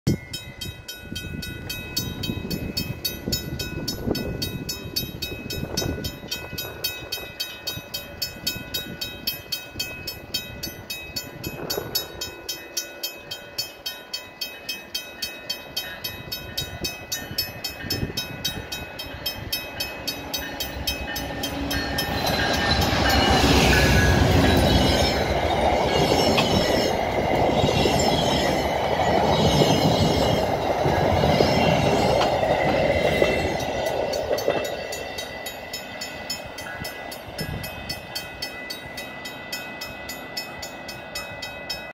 VIA #915 as unknown train number passing through Pleasant Park near the Riverside Hospital.
Filmed in March 4th 2020. Seen in Ottawa Ontario Canada. VIA #915 as unknown train number from Toronto is heading east to Tremblay station.